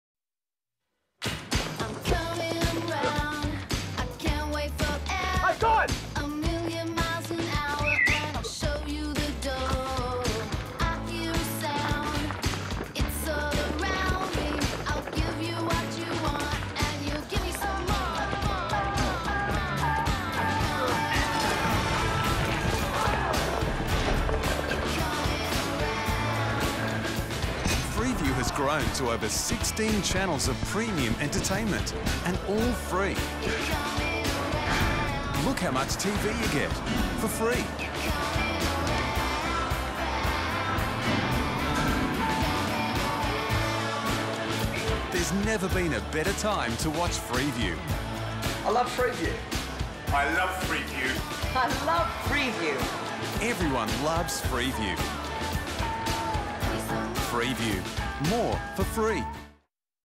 Freeview Promo - 2011
When I did work placement at 44 Adelaide, some volunteers at the time commented with displeasure on the limited coverage 44 Adelaide received in this promo. I wouldn't blame them. (44's logo is on the very top right corner, shown only 15px tall when viewed at 576p. Many bad 'bob' deinterlacers on older tvs halved that, making the logo an unidentifiable red blob.)
Captured from NWS8 2/10/11